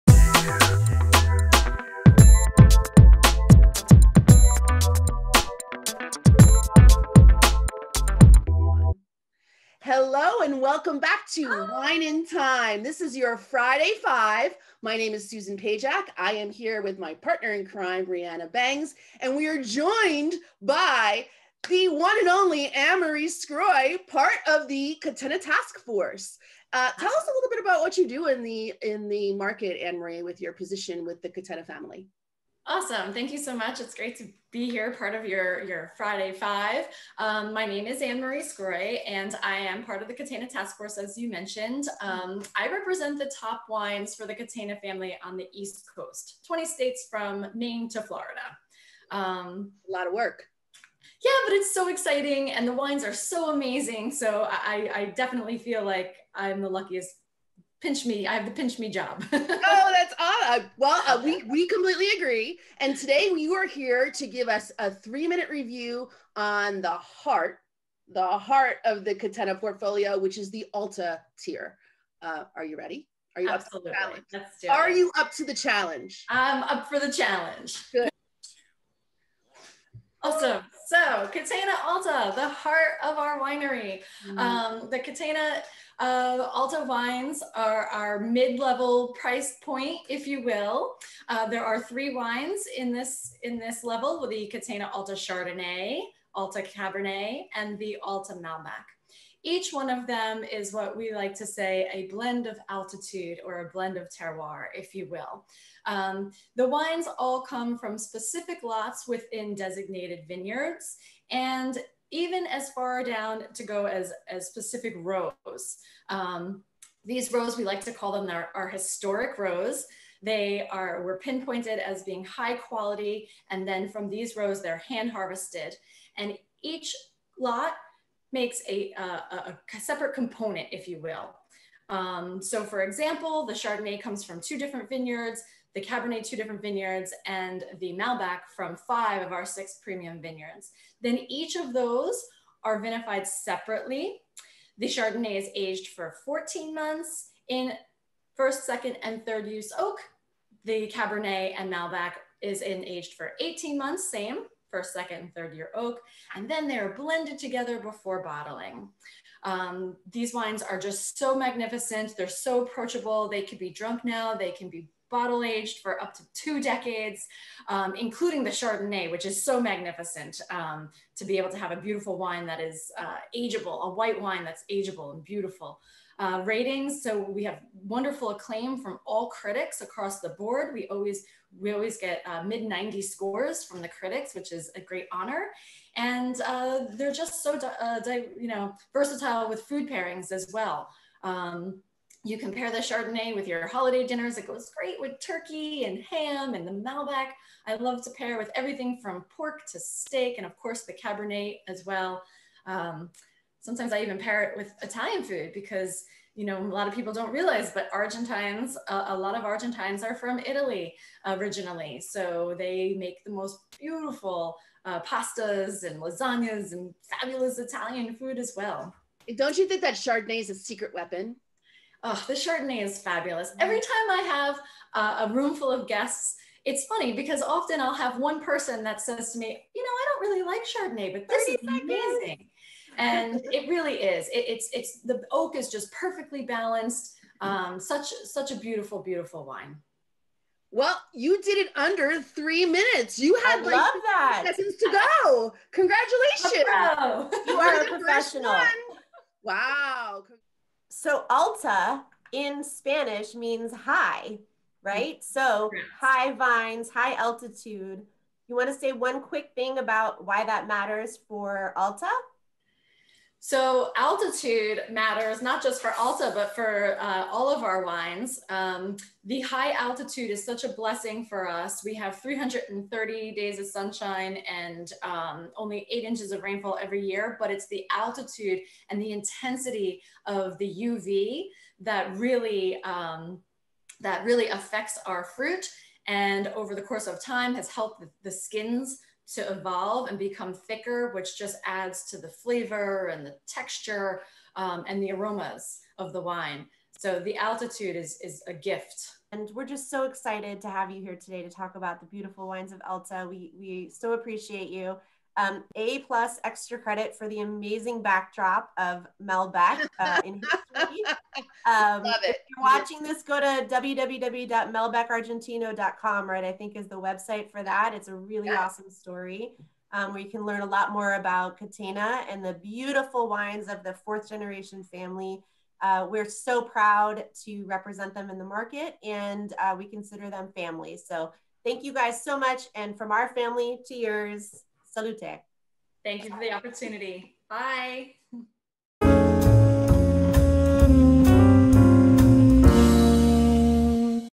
The Friday Five - Catena Alta Review
Challenge: Create and Execute an impactful wine presentation in under three minutes. Can we do it? This week - Rhianna Bangs and Susan Pajak are joined by Ann Marie Sgroi; representative of the winery Bodegas Catena Zapata, from Argentina, for the US Market.  Can Ann Marie successfully outline the Catena Alta tier of wines in three minutes? You be the judge...

In 1994, Nicolás Catena made the first premium varietal Malbec to be exported world-wide from Argentina. It took fifteen years of vineyard research and three years of experimentation at the winery before Nicolas and daughter Laura could decide to bottle the first Catena Malbec. Over the next two years, they identified their best vineyard rows of Cabernet Sauvignon, Chardonnay and Malbec and made the first vintages of the Catena Alta wines in very small quantities. Catena Alta continues to be a limited production of single varietal wines made from a selected few rows in the Catena family's vineyards. (Chardonnay, Cabernet Sauvignon, and Malbec). These special barrels undergo a rigorous second selection at the winery. The Catena Alta wines can usually be drunk starting at three years after harvest and into the following one to two decades.

Or find them on Instagram! @catenawines